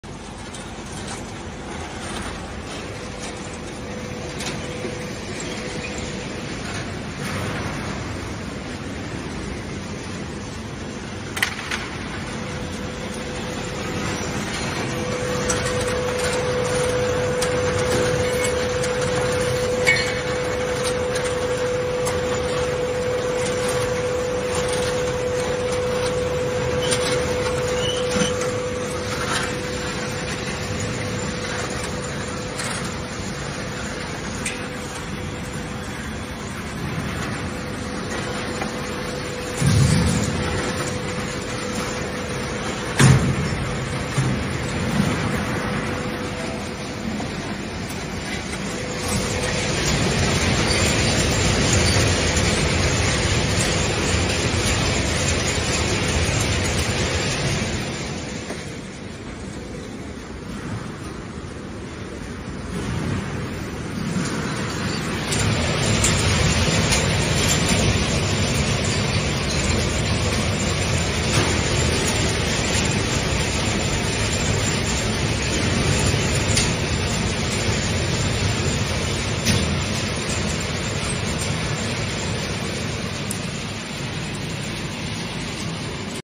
Basket washing and cleaning production line
Suitable for different baskets, vegetable baskets, poultry baskets, fruit baskets and more. Fully sterilize and clean.